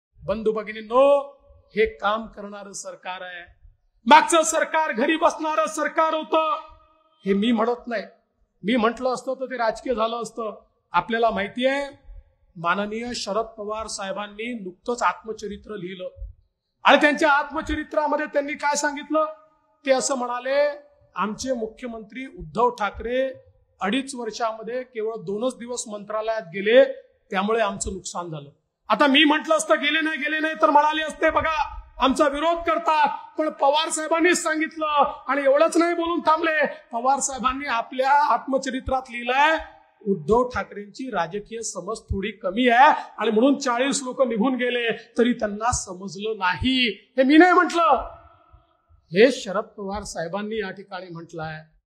मविआचे सरकार घरी बसणारे सरकार! हे खुद्द शरद पवारांनी म्हटले... | अकोला | DCM Devendra Fadnavis
(Modi@9 Grand Public Meeting, Akola | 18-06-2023)